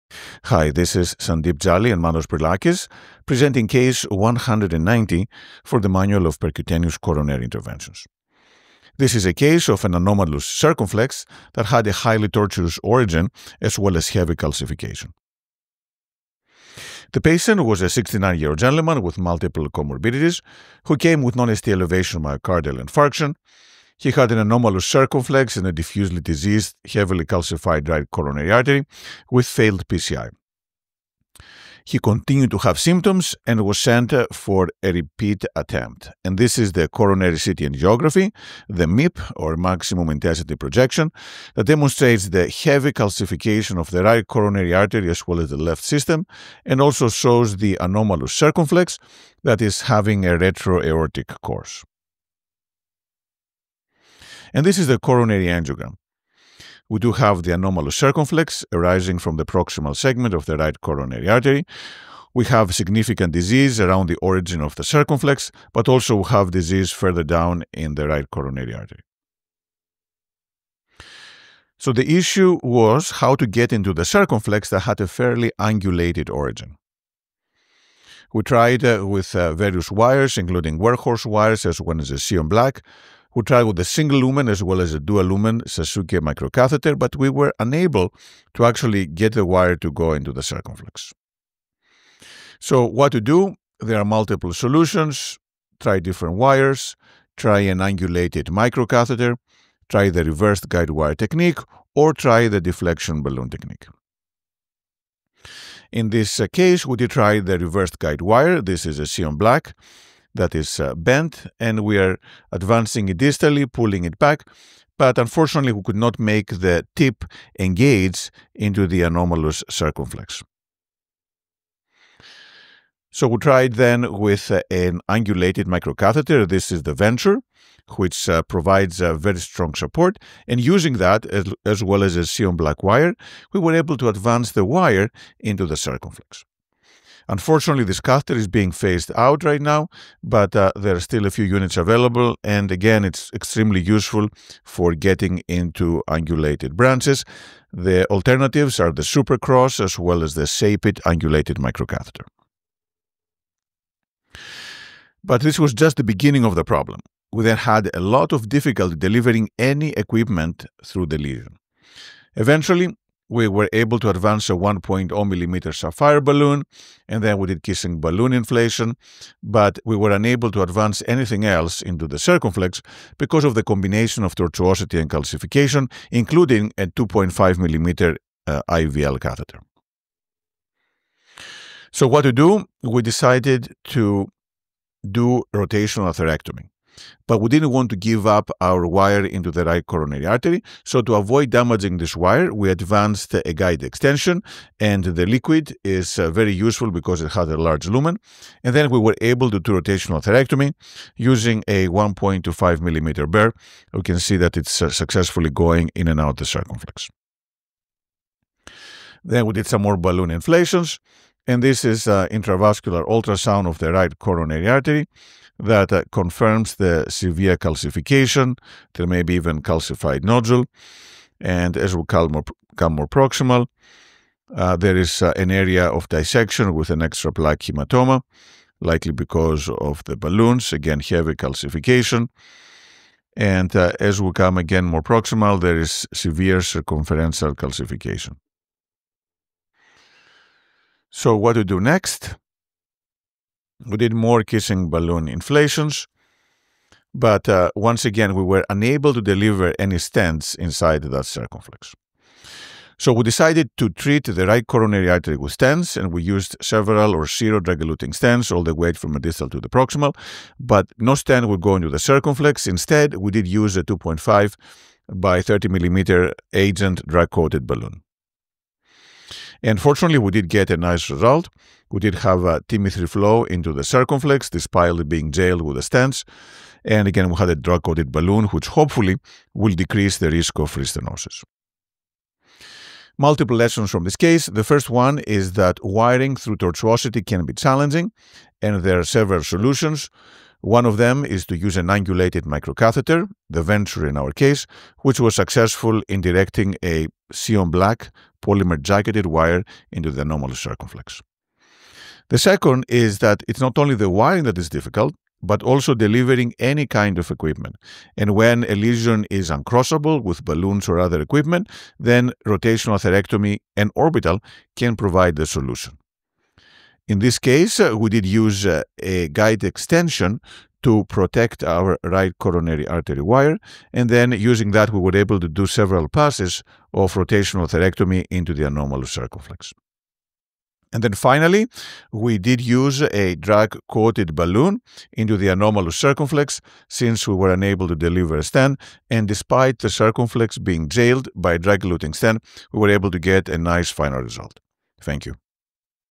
Case 190: Manual of PCI - Heavily calcified + tortuous anomalous circumflex
A patient was referred for PCI of a heavily calcified right coronary artery and an anomalous circumflex arising from the proximal RCA after a failed attempt for wiring the circumflex. Wiring the circumflex was very challenging  because of highly angulated origin. Use of a Sion black wire through a Sasuke microcatheter failed, as did the reversed guidewire technique. Using a Venture angulated microcatheter and a Sion black wire the anomalous circumflex was eventually wired but balloon delivery was extremely challenging: only a 1.0 mm Sapphire Pro balloon could cross. Other balloons and an intravascular lithotripsy catheter could not cross. Rotational atherectomy was performed in the circumflex through a Liquid guide extension so  as not to remove the RCA wire. Stents could still not be delivered into the circumflex because of the tortuosity and calcification. Eventually the anomalous circumflex was treated with a 2.5x30 mm Agent DCB, followed by stenting of the RCA with Orsiro stents with an excellent final result.